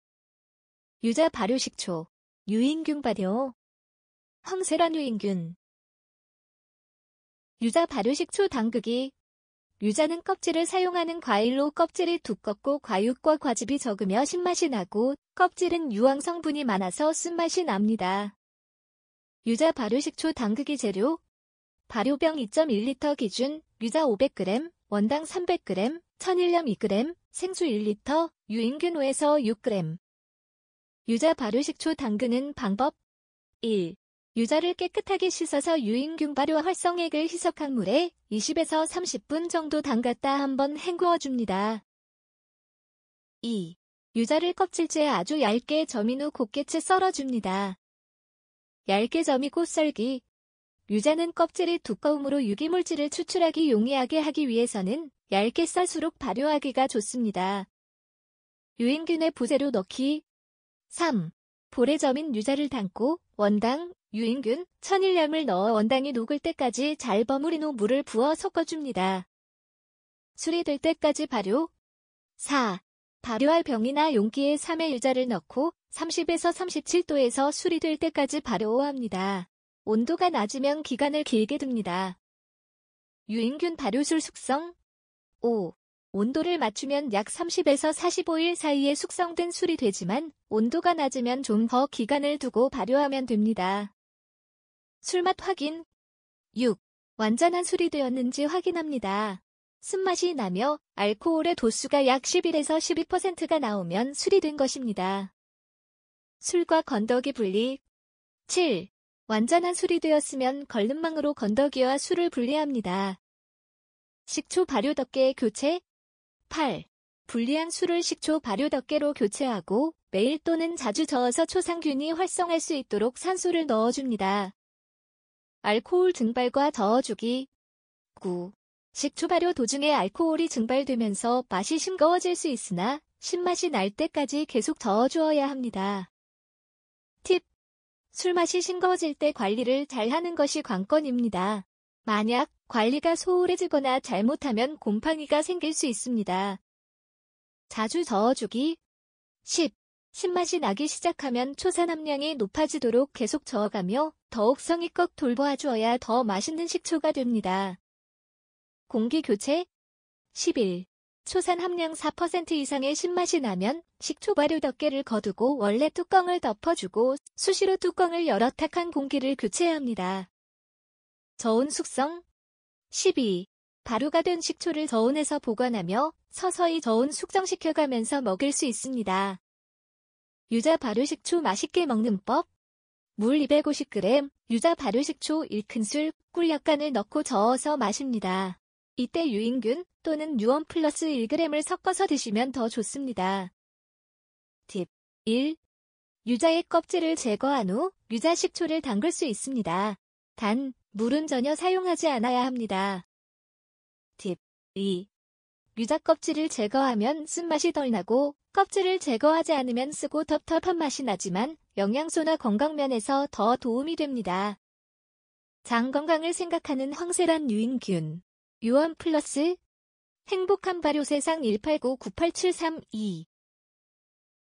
유자발효식초. 유인균, 유원플러스 발효로 식초담그기
유원플러스. 유인균발효식초
유인균과 유원플러스를 활용하여 유자발효식초 담그기입니다.